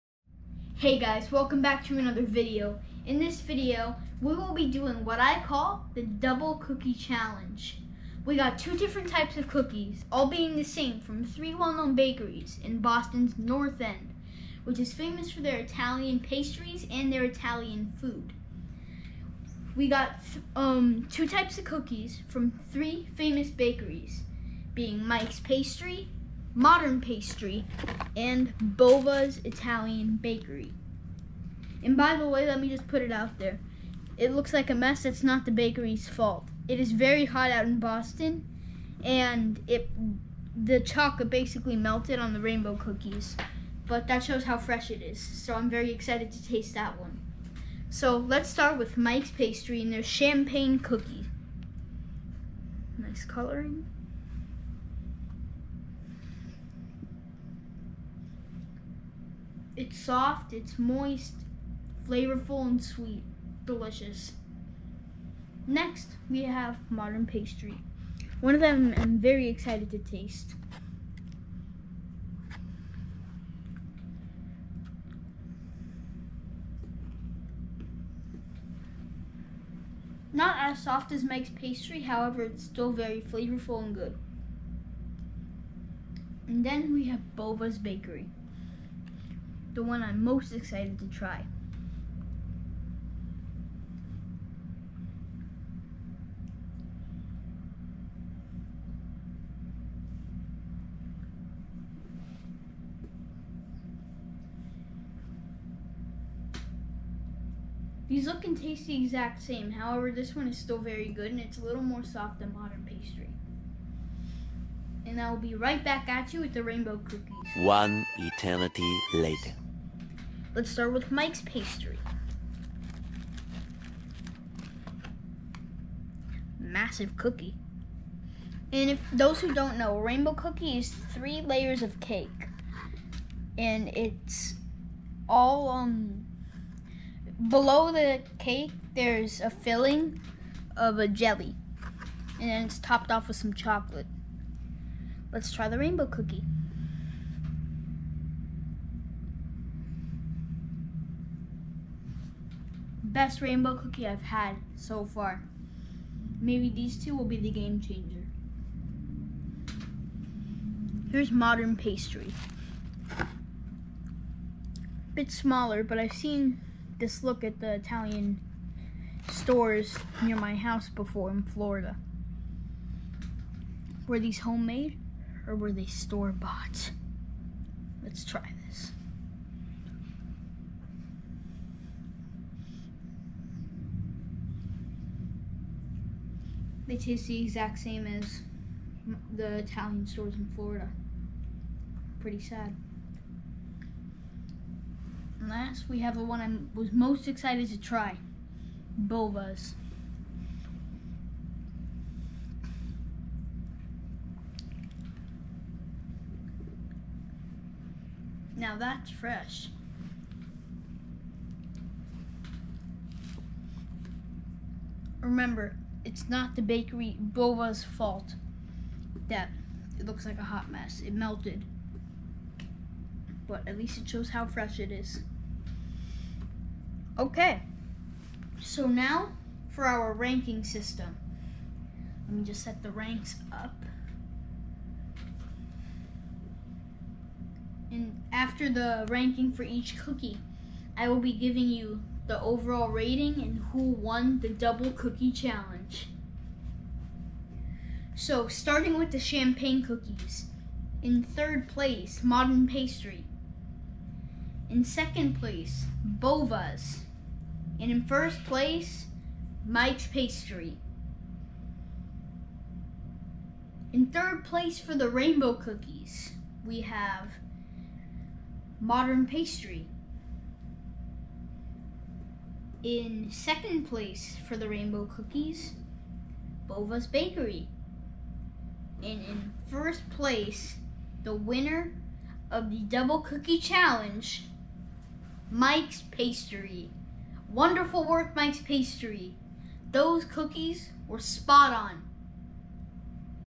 Double Cookie Challenge, Boston MA
Sorry for the long wait! There was a problem exporting the video from the editing program. It finally worked! In this video, we will order two types of cookies from 3 different bakeries. The two cookies we will be trying are the rainbow cookie, which is three different color layers of cake filled with jam and covered with chocolate, and the champagne cookie, which is a shortbread cookie dyed different colors. (not really champagne, I am not old enough). The three bakeries we will be Bova's Italian Bakery, Modern Pastry, and Mike's pastry, all of which are in Boston's North End, which is famous for the Italian food, and pastries. Watch the video to see who wins, and where to go on your next visit to Boston!